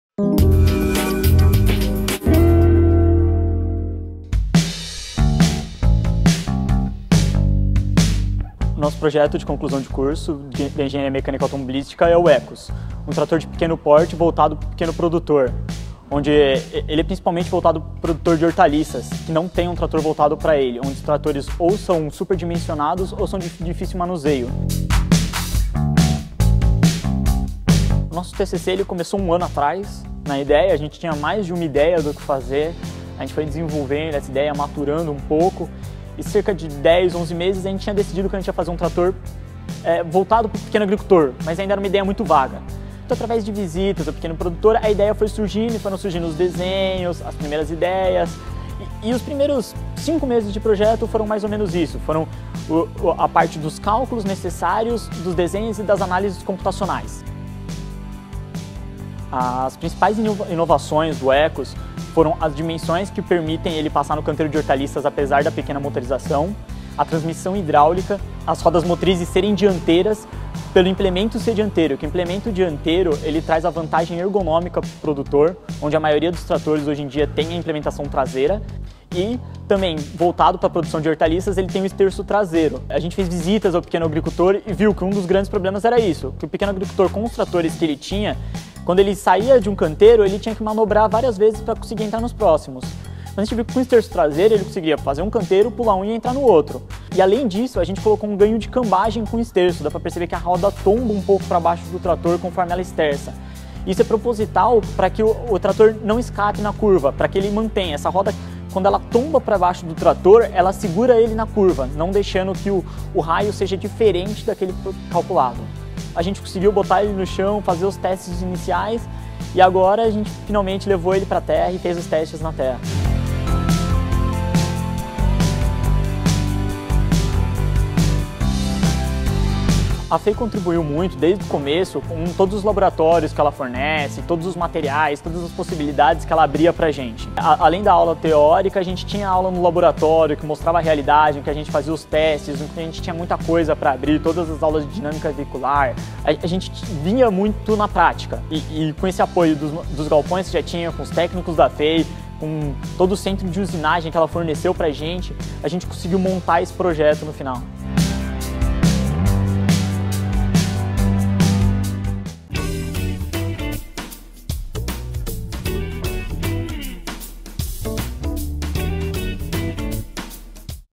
Alunos de Engenharia da FEI desenvolvem Mini Trator Eqüus para otimizar a agricultura
Desenvolvido por alunos de Engenharia Mecânica da FEI, o Eqüus é um mini trator que tem como promessa otimizar o tempo e garantir melhores condições físicas de trabalho ao pequeno agricultor. Neste vídeo, o aluno Enzio Bianchini explica como foi o processo de desenvolvimento e apresenta o trator em movimento na terra.

Saiba mais sobre o projeto, também, no Blog da FEI: